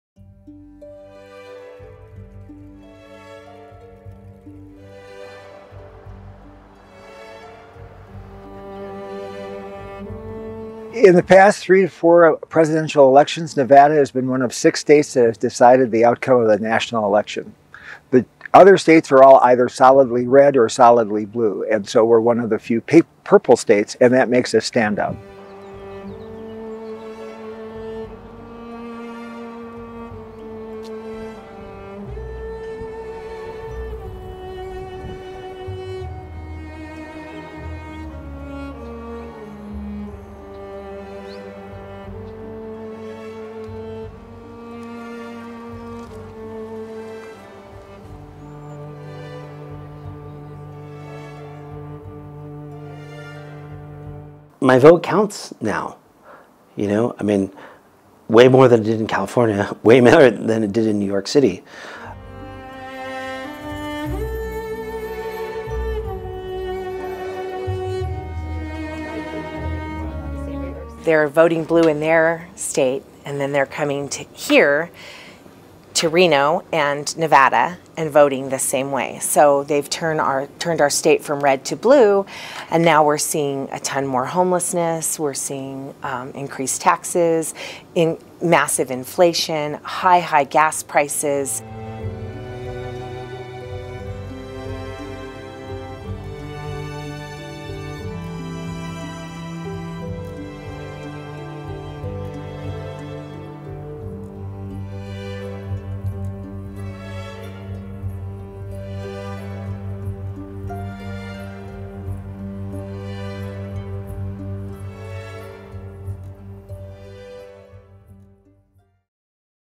Au Nevada, la relocalisation des californiens pourrait modifier l'élection américaine
L'augmentation récente de la migration des Californiens vers le Nevada pour y trouver de meilleurs emplois et un coût de la vie moins élevé pourrait avoir un impact sur les prochaines élections présidentielles, et les électeurs républicains du Nevada n'en sont pas très heureux.